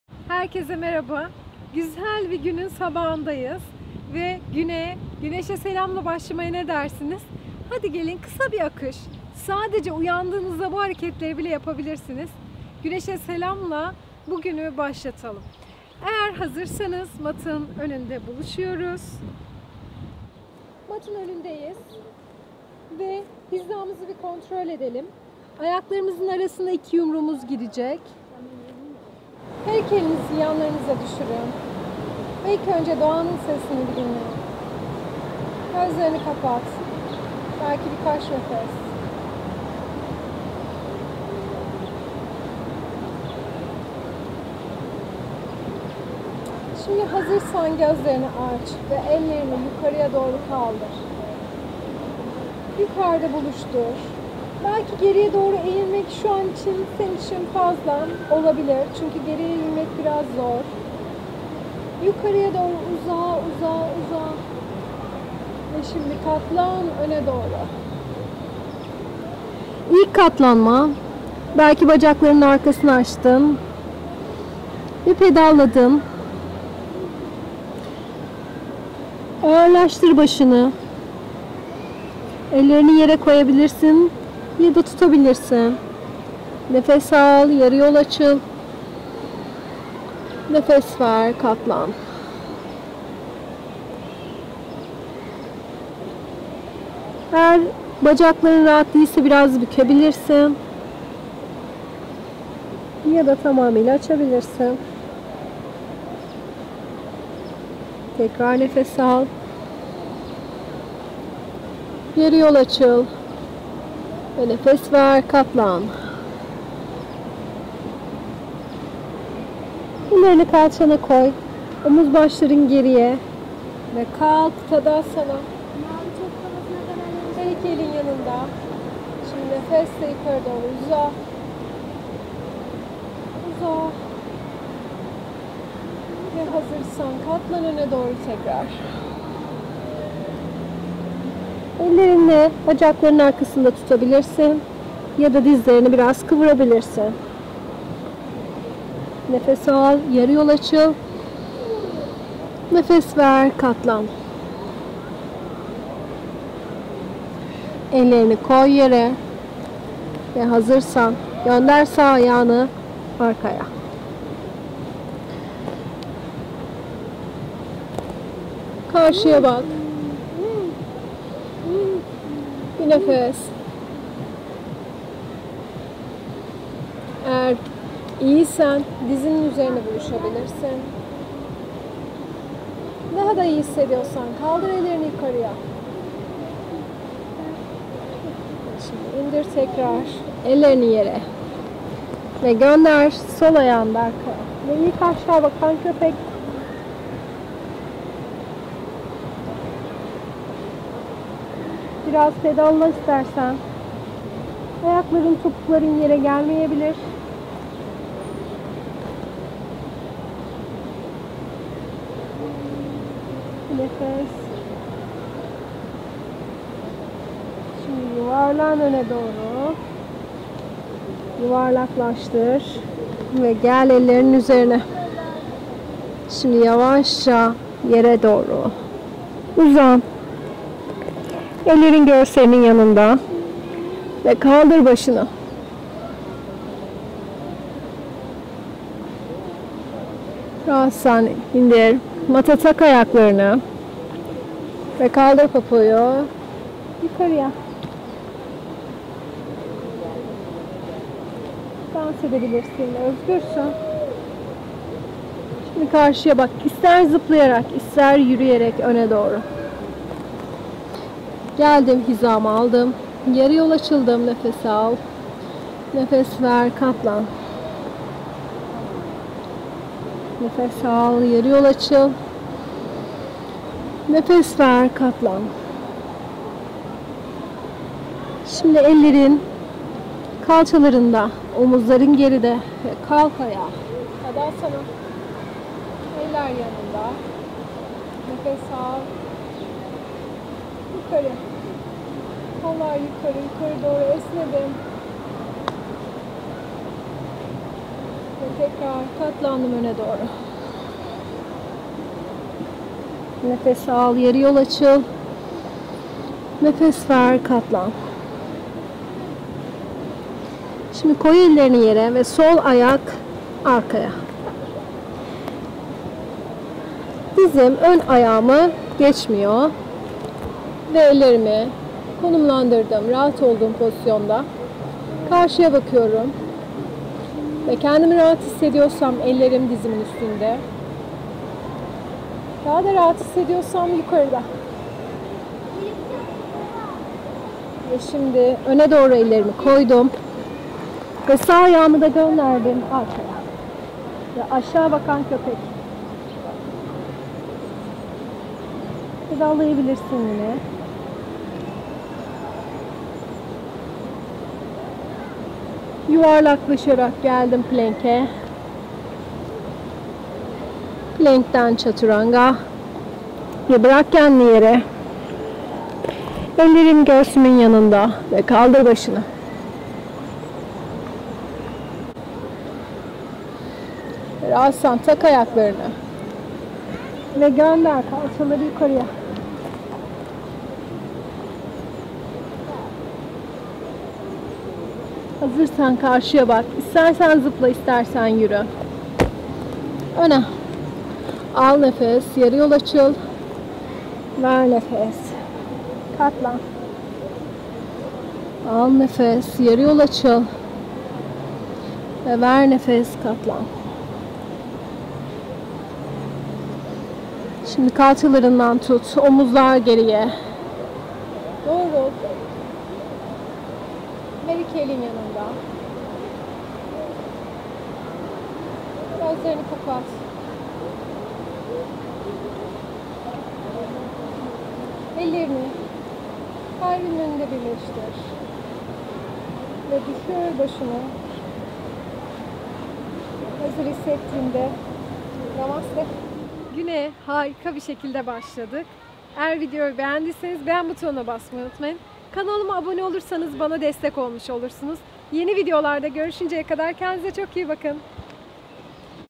Güneşe selam, yoga başlangıç, doğada güne başla , sabah sporu, hareketle başla, doğa sesleriyle yoga
Sabah yogasıyla güne başlamaya ne dersin. Birlikte yapmamın şükrü ile 🙏 doğanın sesini dinle , 7 dk yoga .